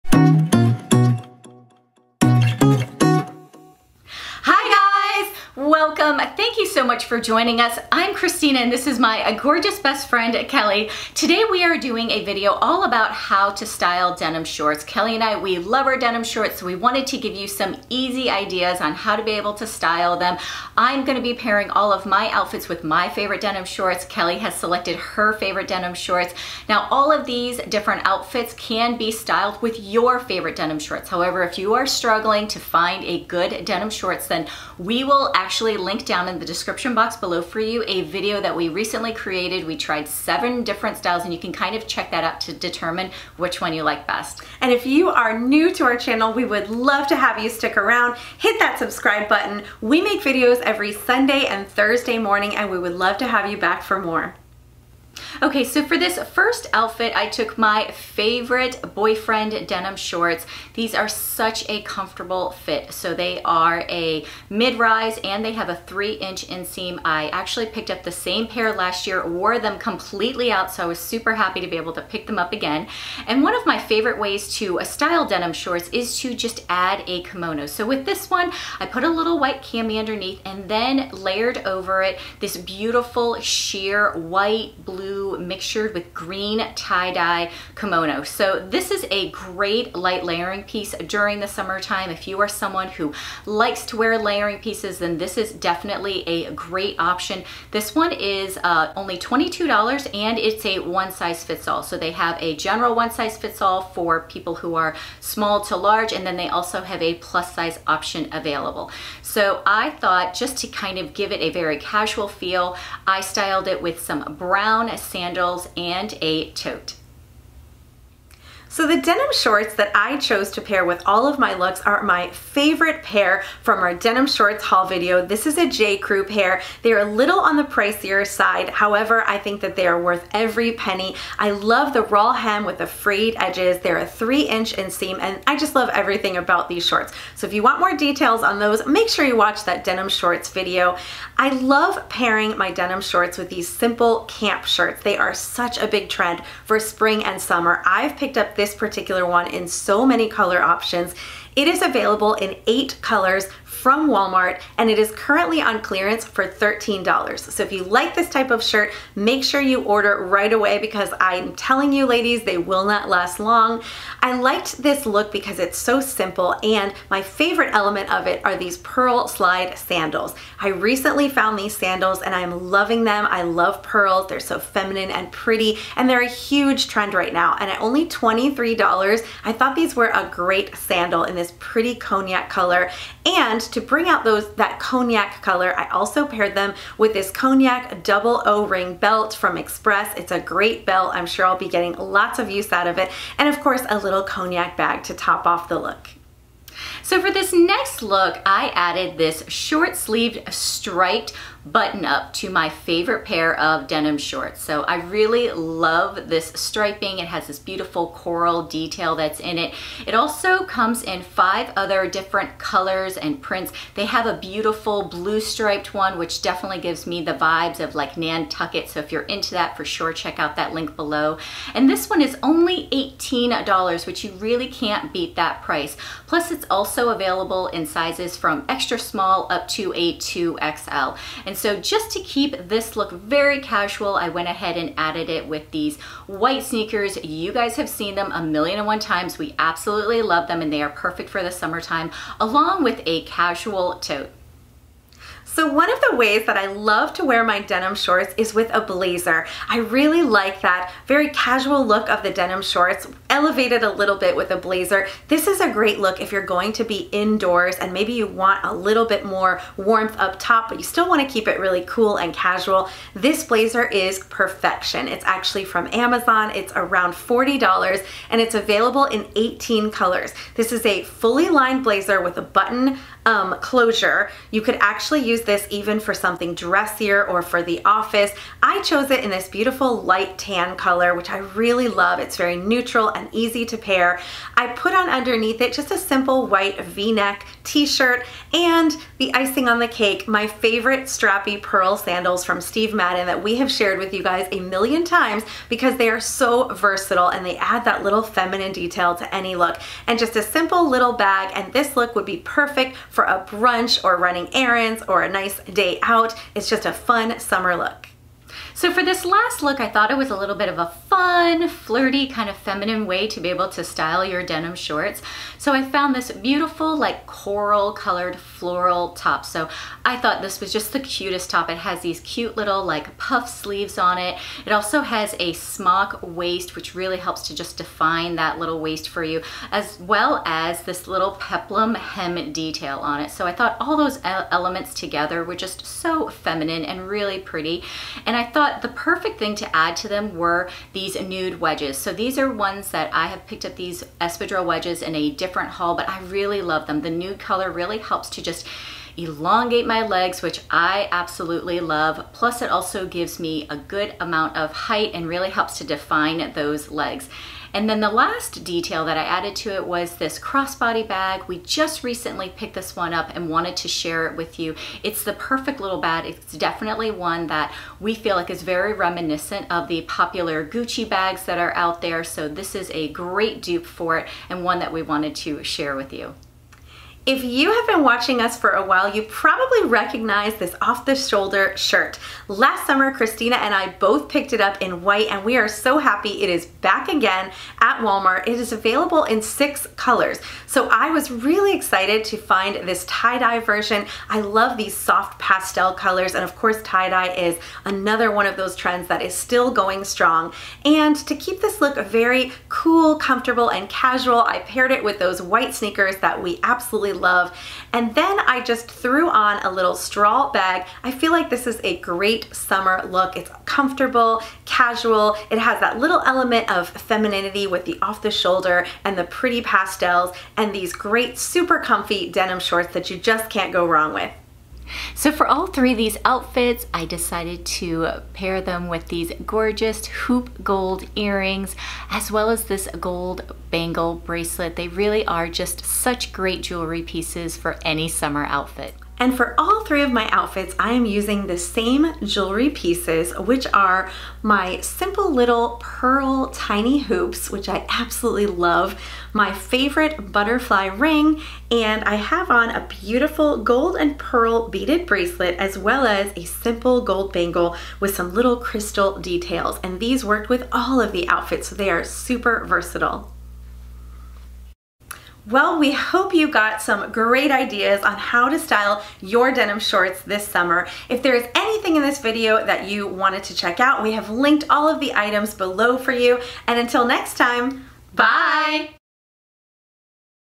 Denim Shorts for Summer | 6 Outfit Ideas with Jean Shorts for Women Over 40
Hi ladies! Summer is almost here and we are so ready to break out the denim shorts outfits. We have put together 6 adorable outfit ideas using our favorite pairs of jean shorts. All of these casual, comfortable outfits are perfect for women over 40. We have linked all of the items shown below in case you are interested.

Christina's Intro Top (M)
Christina's Jewelry (worn throughout the video)
Large Gold Hoops
Gold bangle

Kelli's Intro Top:(M)
Kelli's Jewelry (worn throughout the video)
Small Pearl Hoops
Butterfly Ring
Pearl Bead Bracelet
Bangle Bracelet

Outfit #1:
White tank top (L)
Denim shorts:(size 10-orchid)
Brown Sandals:(8)
Straw Circle Bag:(different strap)

Outfit #2:
Camp Shirt:(S)
Cognac Belt:(S)
Denim shorts:(size 27-squash wash)
Cognac Pearl Sandals:(8)

Outfit #3:
Striped Shirt:(M-coral)
Denim shorts:(size 10-orchid)
White Sneakers:(8)
Tote Bag:(English saddle)

Outfit #4:
Tan Blazer:(M-sized up one/khaki)
V-neck tshirt:(S)
Pearl strappy sandals:(8)

Outfit #5:
Puff sleeve top:(M)
Denim shorts:(size 10-orchid)
Espadrille Wedges:(8)
Crossbody bag:(light brown)

Outfit #6:
OTS top:(S)